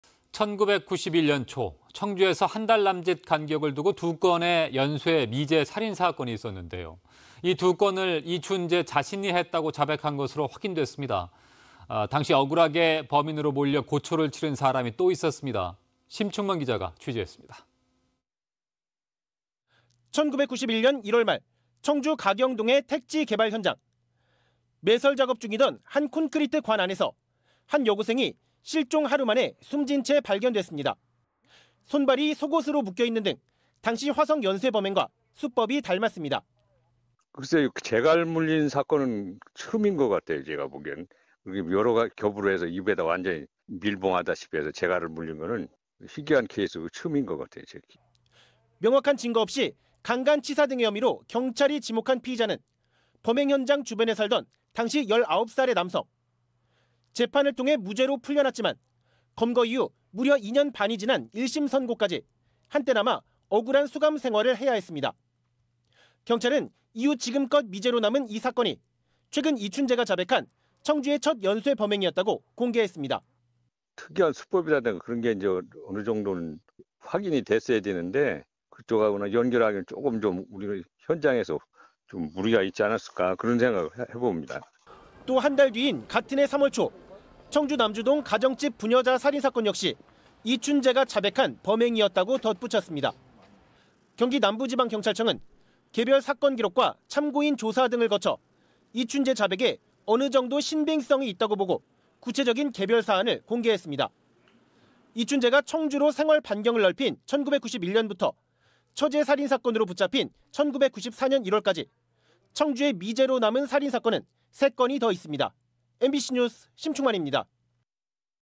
청주 연쇄살인도 이춘재, "억울한 누명 또 있었다"ㅣMBC충북NEWS
[앵커]
 1991년 초 청주에서 한달 남짓 간격을 두고
두 건의 연쇄 미제살인사건이 있었는데요,
 이 두 건을 이춘재 자신이 했다고
자백한 것으로 확인됐습니다.
 당시 억울하게 범인으로 몰려 고초를 치른
사람이 또 있었습니다.
 심충만 기자입니다.
         [기자]

 1991년 1월 말 청주 가경동의 택지개발 현장.

 매설 작업 중이던 한 콘크리트관 안에서
여고생이 실종 하루 만에 숨진 채 발견됐습니다

 손발이 속옷으로 묶여 있는 등
당시 화성 연쇄 범행과 수법이 닮았습니다.

   [이성기 전 경위 / 당시 감식 경찰관]
 "재갈 물린 사건은 당시 처음인 것 같아요, 제가 보기엔. 여러 겹으로 해서 입에 완전히 밀봉하다시피 해서 재갈을 물린 것은 희귀한 케이스고 처임인 것 같아요"

 명확한 증거 없이 강간치사 등의 혐의로
경찰이 지목한 피의자는
범행현장 주변에 살던 당시 19살의 남성.

 재판을 통해 무죄로 풀려났지만,
검거 이후 무려 2년 반이 지난 1심 선고까지
한때나마 억울한 수감 생활을 해야 했습니다.

 경찰은 이후 지금껏 미제로 남은 이 사건이
최근 이춘재가 자백한
청주의 첫 연쇄 범행이었다고 공개했습니다.

 "특이한 수법이라든가 그런 것이 어느 정도 확인이 됐어야 하는데 그쪽(화성)하고 연결하기는 조금 현장에서 무리가 있지 않았을까 그런 생각을 해 봅니다"

 또 한달 남짓 뒤인 같은해 3월 초,
청주 남주동 가정집 부녀자 살인사건 역시
이춘재가 자백한 범행이었다고 덧붙였습니다.

 경기남부지방경찰청은
개별 사건 기록과 참고인 조사 등을 거쳐
이춘재 자백에 어느 정도 신빙성이 있다고 보고
구체적인 개별 사안을 공개했습니다.

 이춘재가 청주로 생활반경을 넓힌 1991년부터
처제 살인사건으로 붙잡힌 1994년 1월까지,
청주에 미제로 남은 살인 사건은
3건이 더 있습니다.
 MBC뉴스 심충만입니다.